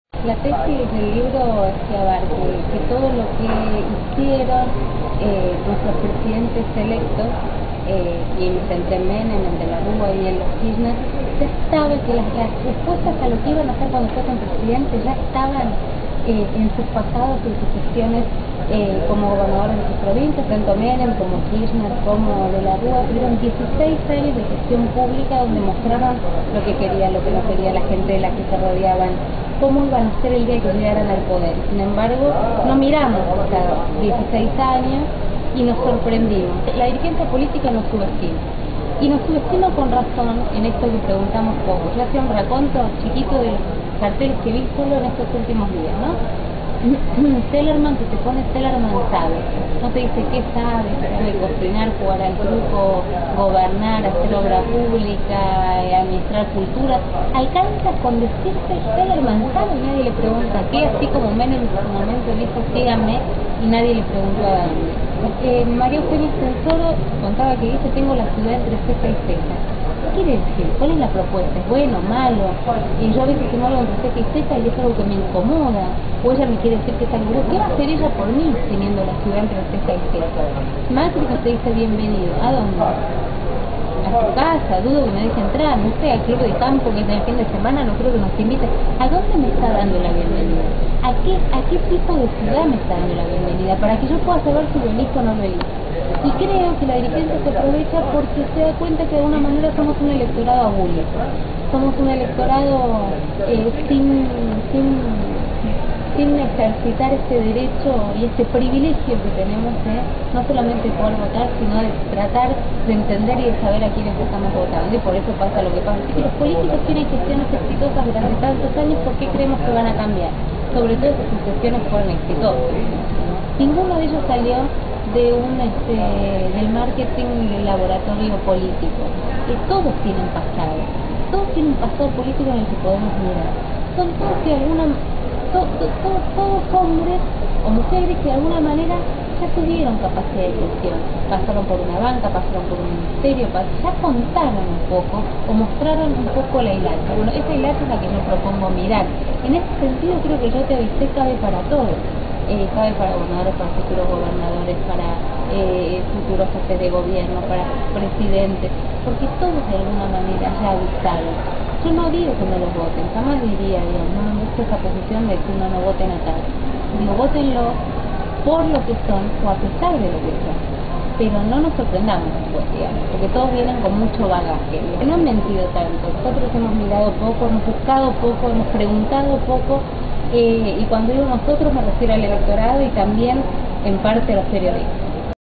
Romina Manguel: "Somos un electorado abúlico"
Analizando el pasado político de los últimos cuatro presidentes argentinos, la periodista Romina Manguel armó un muestrario de los rasgos repetidos en sus gestiones que, según la autora, el electorado no vislumbró.